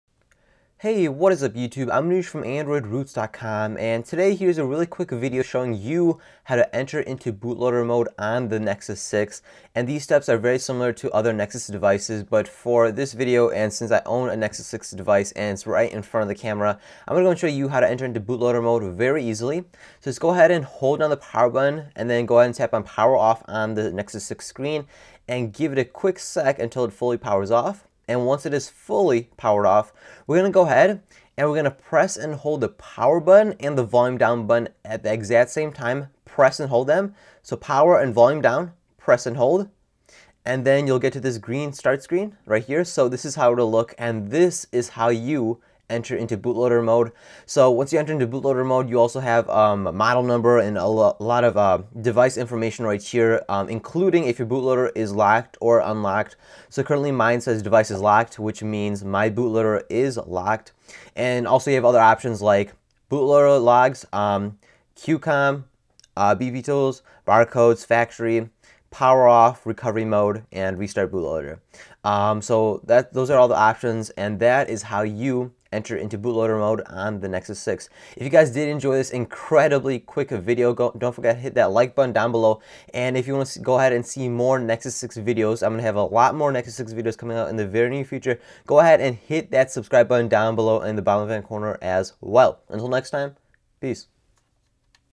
How to Enter Into Bootloader Mode on Nexus 6!
A quick tutorial showing you how to enter into Bootloader Mode on the Nexus 6.

Thank you for all the support you have given me, I truly appreciate it!